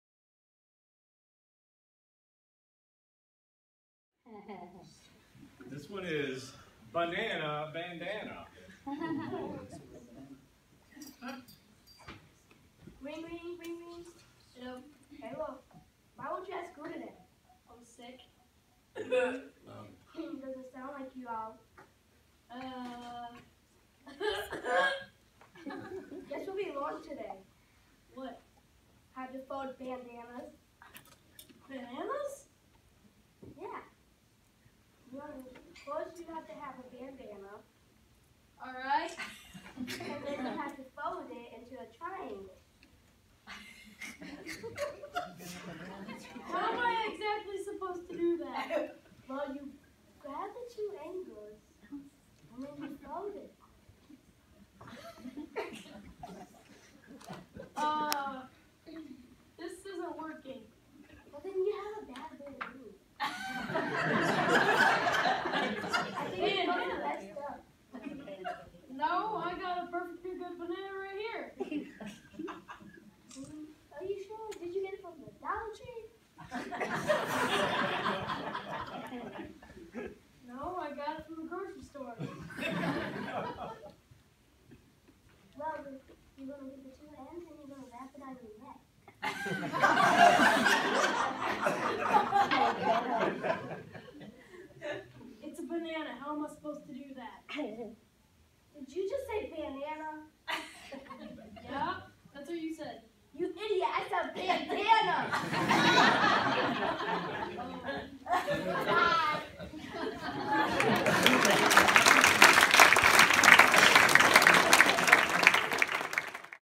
Banana Bandana
One of our youth skits from our 49th Annual Christmas Banquet.